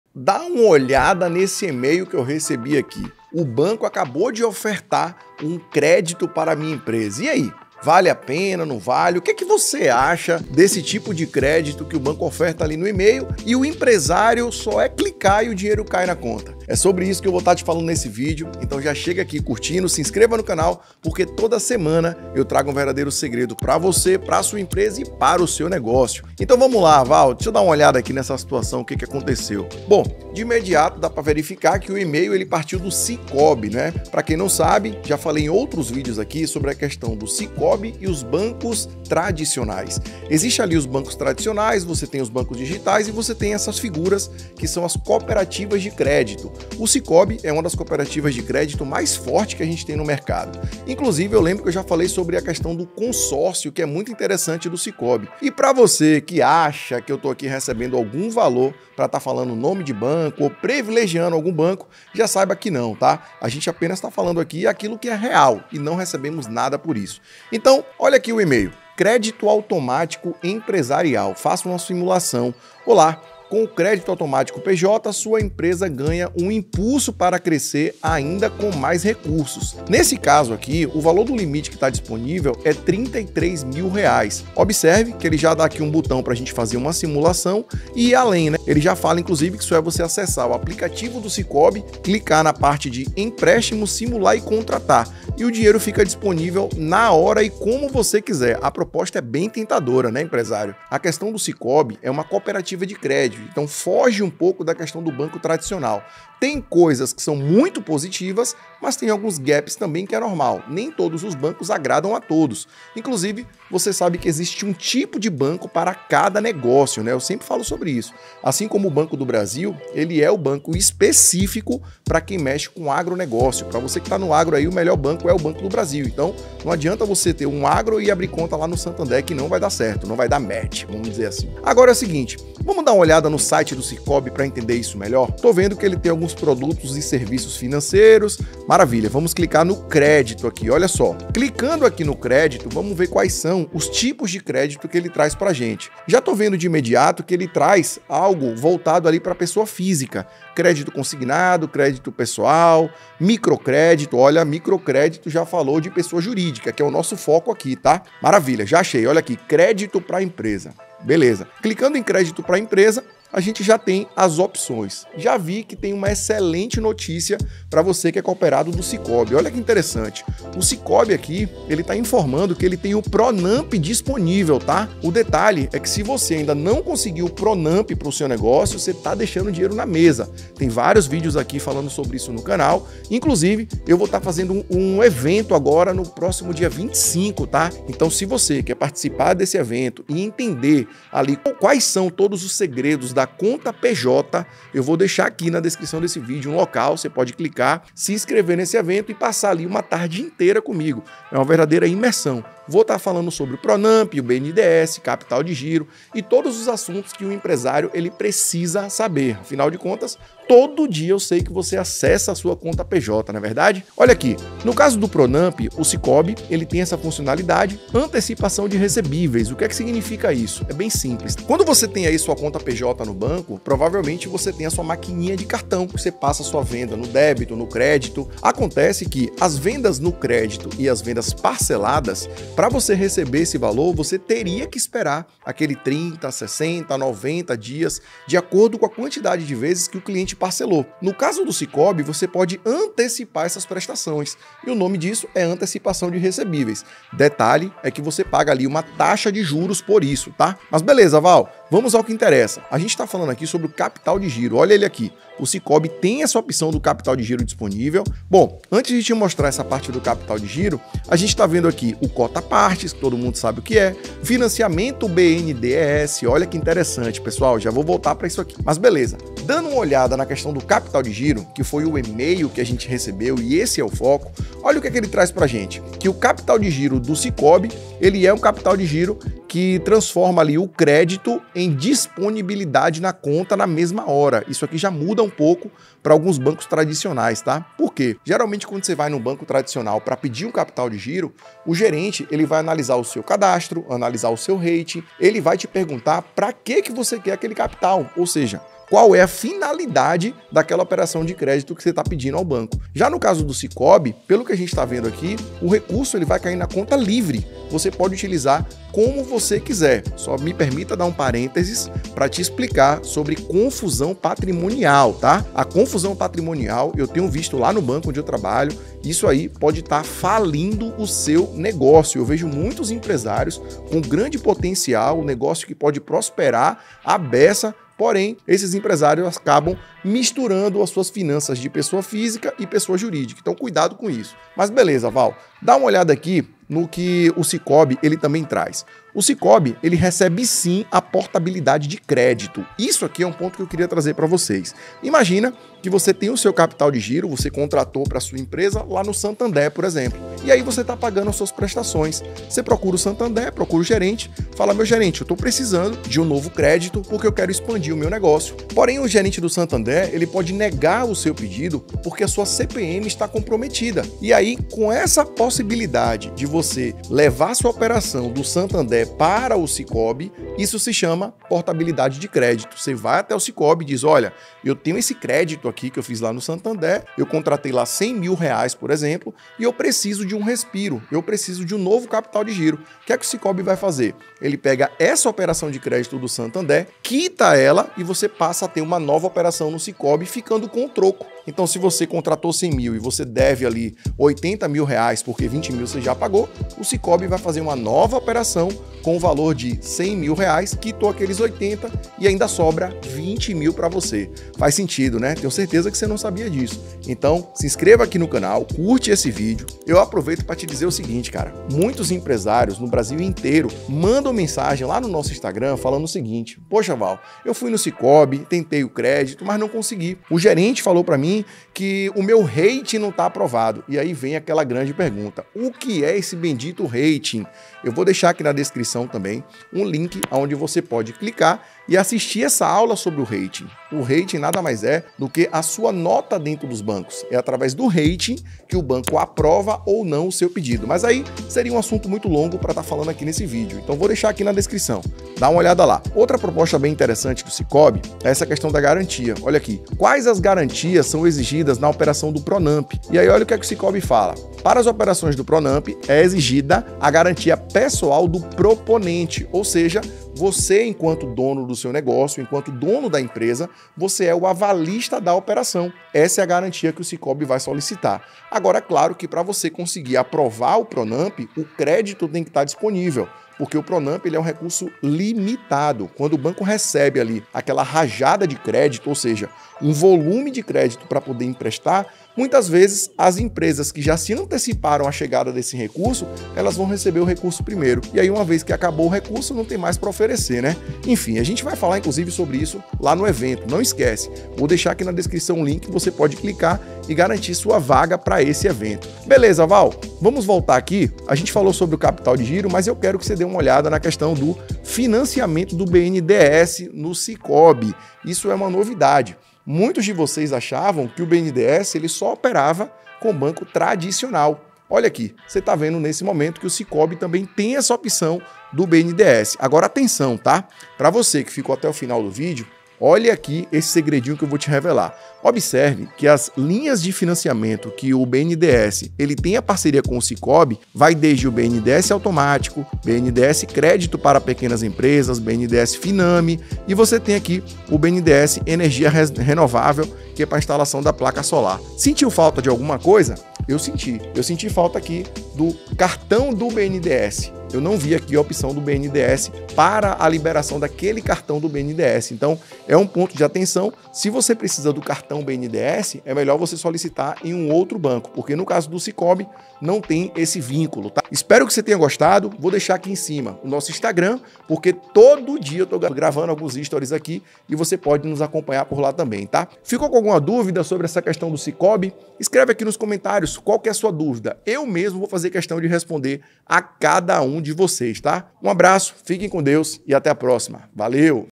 CAPITAL DE GIRO SICOOB - Saiba tudo sobre esse crédito e veja se está DISPONÍVEL para seu NEGÓCIO
✅  Para empresa LTDA, ME e grande empresas: Descubra os Segredos da conta PJ:

✅  Para MEI e pequenas empresas: Descubra como CONSEGUIR CRÉDITO DOS BANCOS RÁPIDO:

Sicoob vale a pena por conta da conta empresa para o empreendedor e muito mais quando o empresário entende a diferença entre o banco tradicional e as cooperativas de crédito.

Ter a conta PJ no banco certo e de acordo ao ramo de negócio é algo que vai mudar completamente a saúde financeira da empresa. A taxa de juros do sicoob comparada com outros bancos vale a pena por ser mais atrativa.

O cooperado quando abre a conta PJ no sicoob recebe o benefício de ser parte da instituição. Por esse motivo ter acesso ao crédito vai ser sempre positivo para o empresário que sabe utilizar a conta PJ.

O melhor banco para fazer o capital de giro do seu negócio será sempre aquele que possuir o melhor relacionamento e a menor taxa de juros na contratação.

Aplica as dicas e vamos juntos,

Abração,

Wal macedo

00:40 Diferença sicoob e banco
01:59 Melhor banco para empresa
02:59 pronampe disponível
03:12 Evento segredos da conta PJ
03:46 Maquina de cartão
05:20 Confusão patrimonial
06:39 Operação de crédito